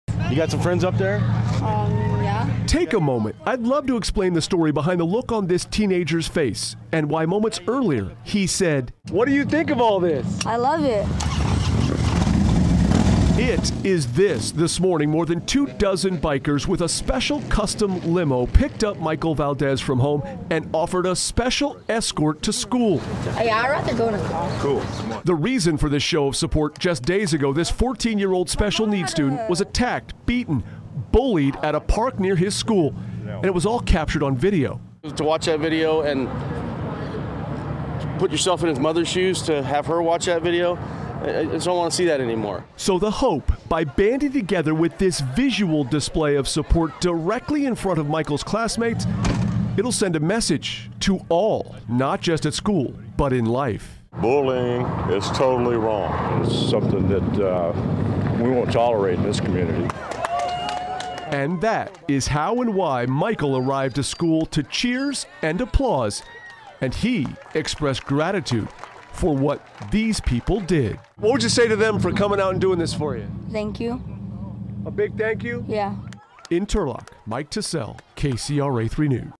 Bullied teen shows up to Turlock school in style with motorcycle escort
Dozens of motorcycles escorted a bullied, special needs teenager riding inside an eye-catching car as it rolled up to school drop-off in Turlock on Tuesday morning. "Seeing his face when he saw all his classmates watching, that was all worth it," said Larry Nydahl, a motorcycle rider with the American Legion Riders of Turlock. Earlier this month, cellphone video captured Michael Valdez, 14, being beaten and bullied at a park near Turlock Junior High School. The video went viral on social media and led to hundreds of calls to police. The school district is investigating. Get the full story in the video above.